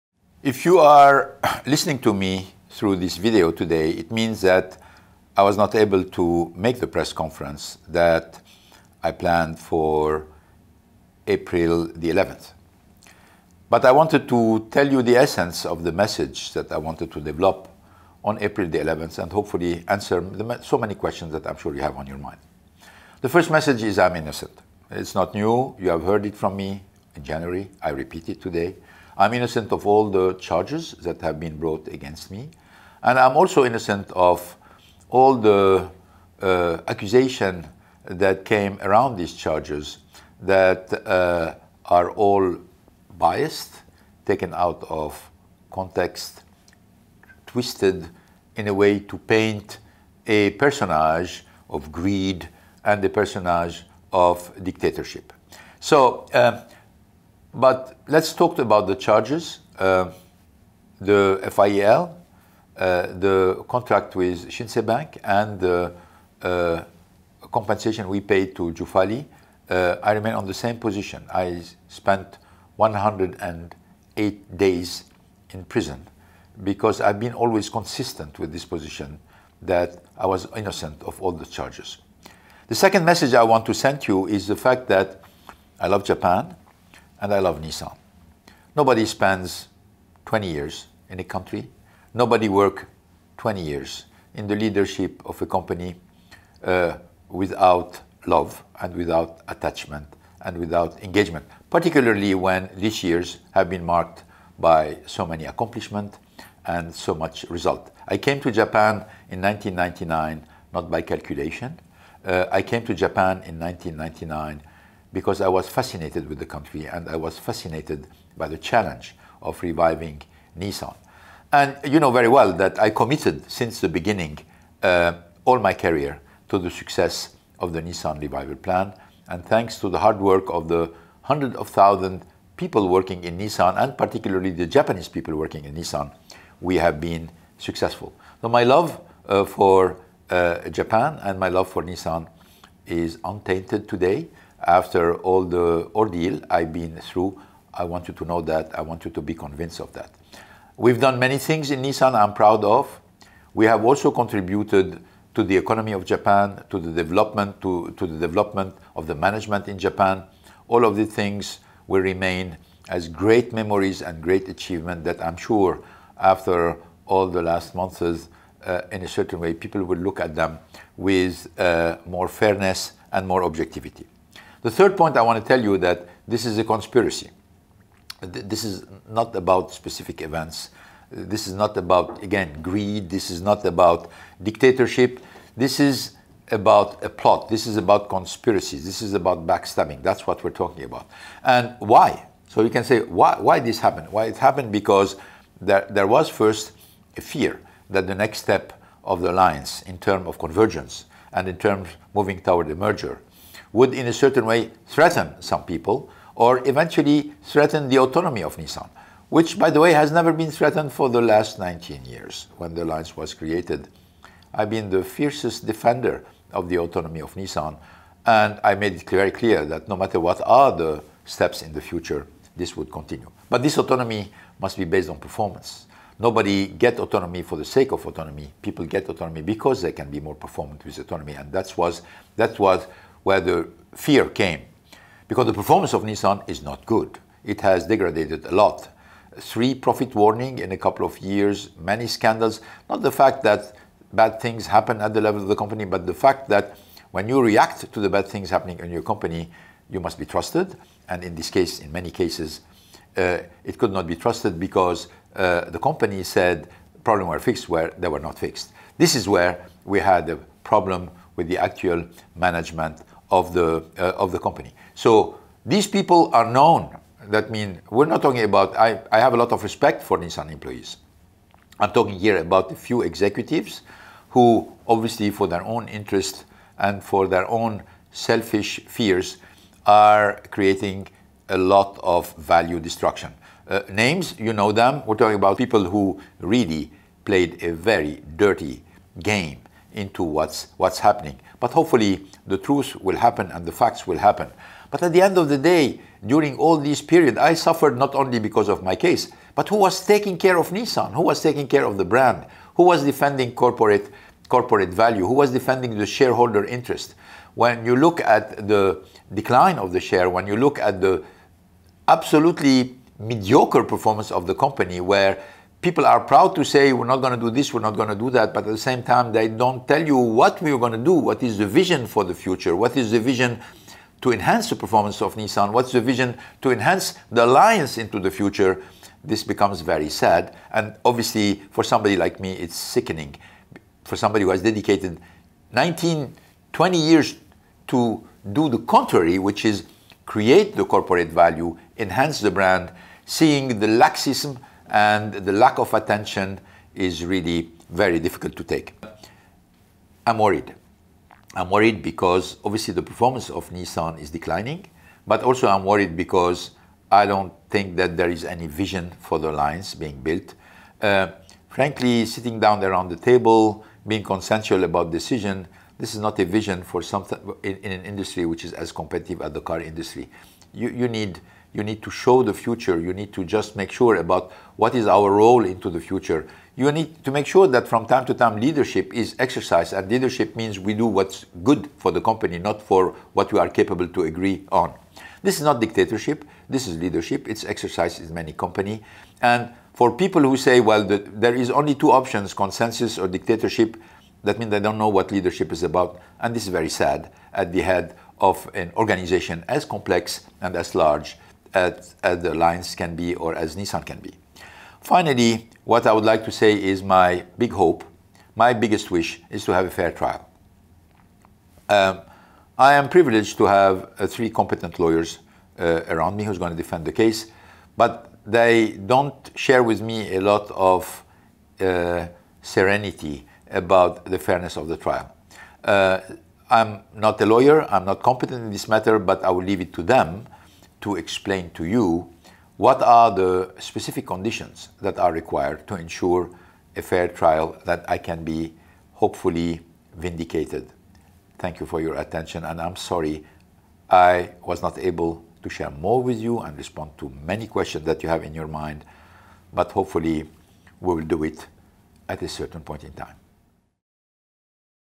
ゴーン前会長の映像公開「すべての嫌疑について私は無実」
日産自動車前会長のカルロス・ゴーン容疑者の弁護団は４月９日、前会長が会社法違反（特別背任）の疑いで４日に再逮捕される直前に収録した映像を公開した。